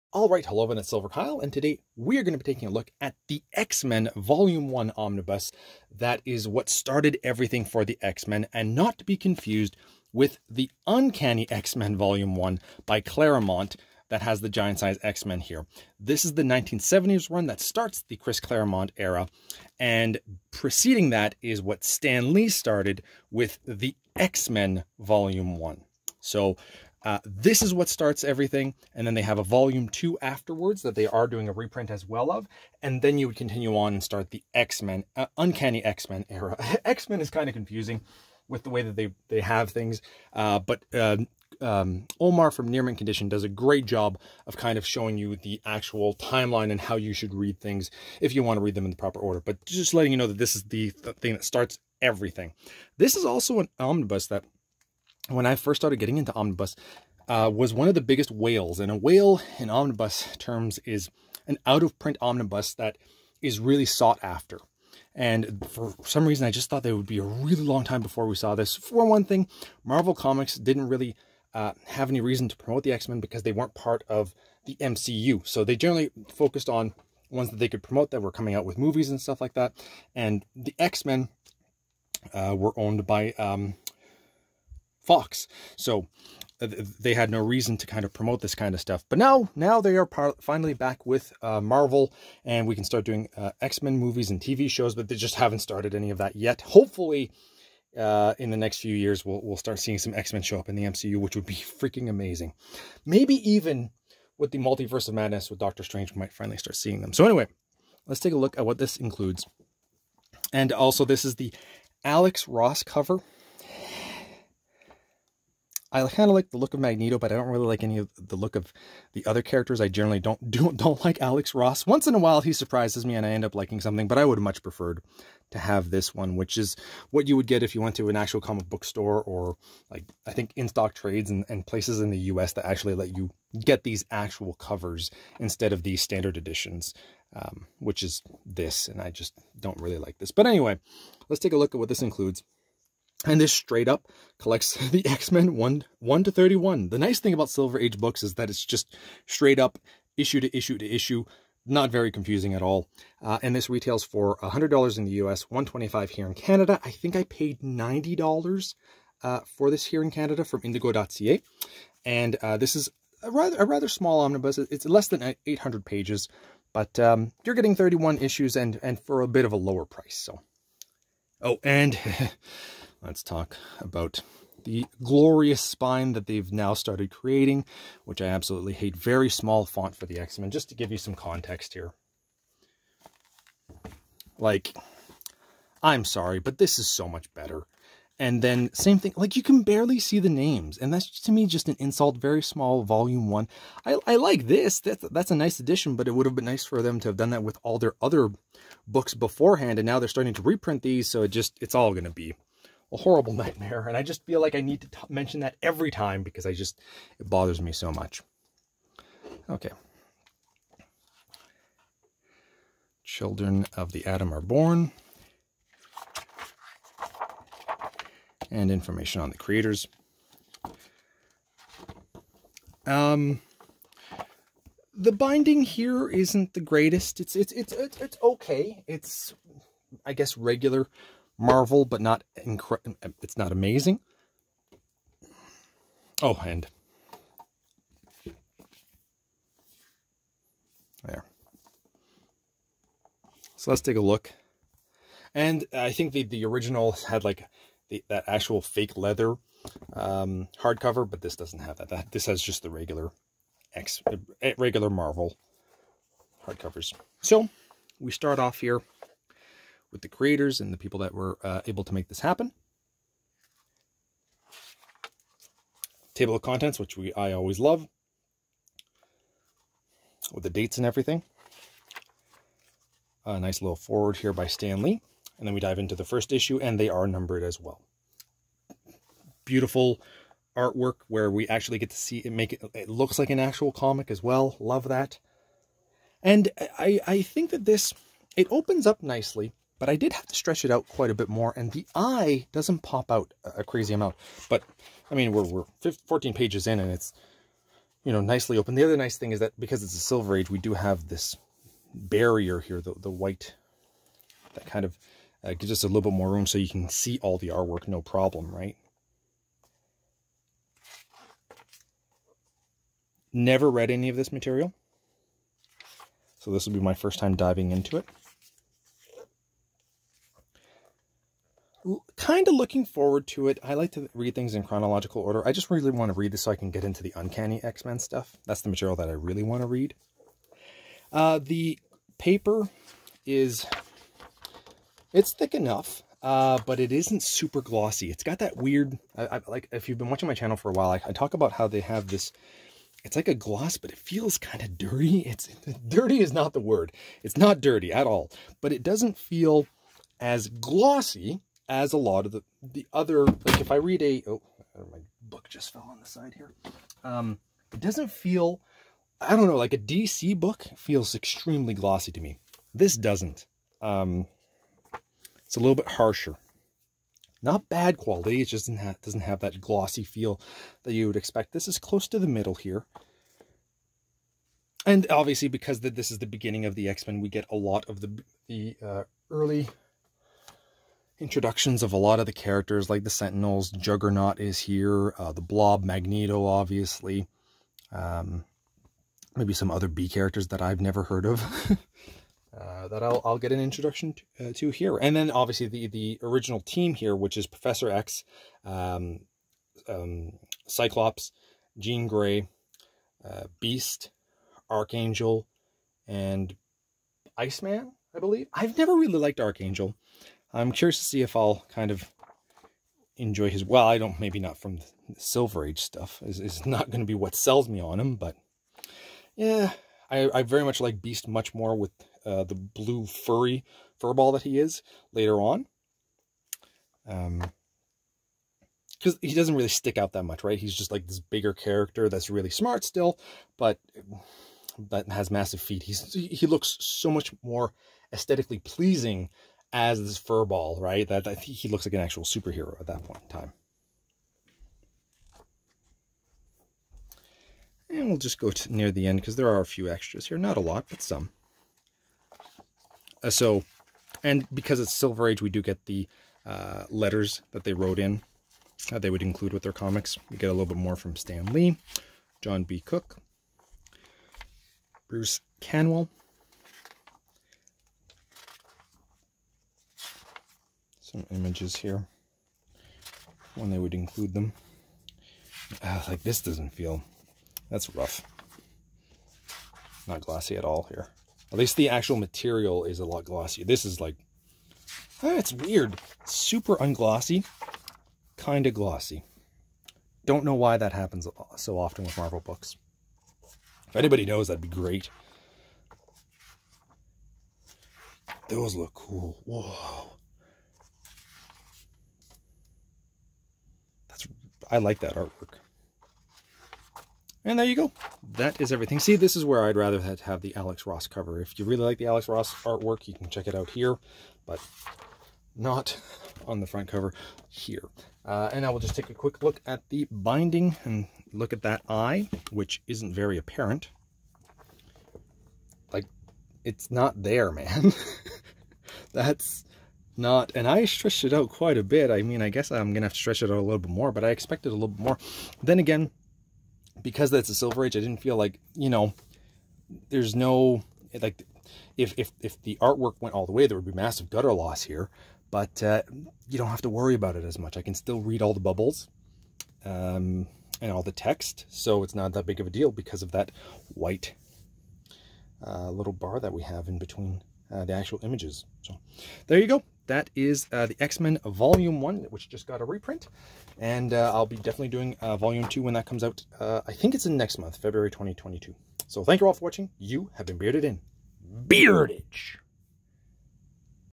The X-MEN Vol. 1 Omnibus Overview - 2022 Reprint
Taking a look at the 2022 Reprint of the X-MEN Vol. 1 Omnibus!